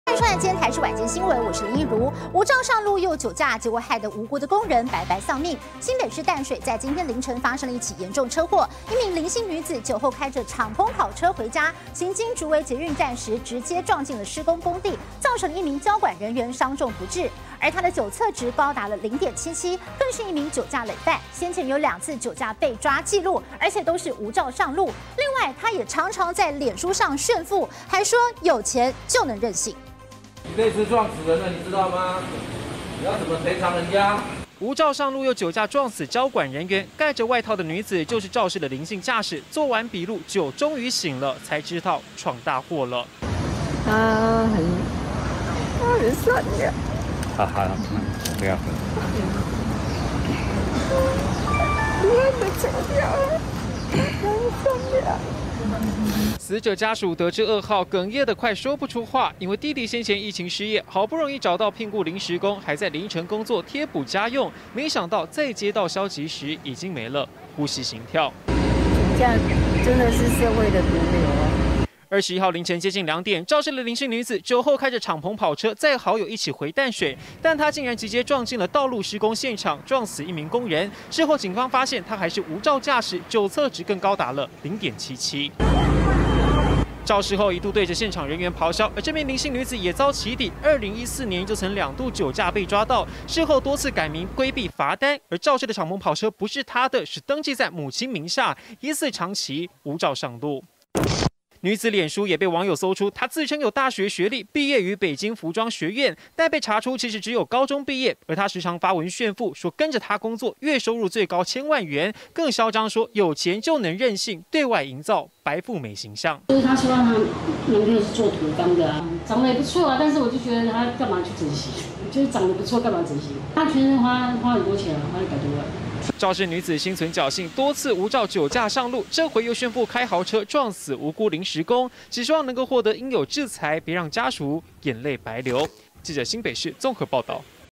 第3度無照酒駕! 跑車炫富女撞死人 移送地檢
無照上路又酒駕，害得無辜工人白白喪命。新北市淡水今天(21日)凌晨發生嚴重車禍，一名林姓女子酒後開著敞篷跑車回家，但行經竹圍捷運站時，直接撞進施工工地，造成一名交管人員傷重不治，肇事駕駛酒測值高達0.77。她更是一名酒駕累犯，先前也有2次酒駕被抓紀錄，而且都是無照上路，另外她也時常在臉書炫富，說有錢就能任性。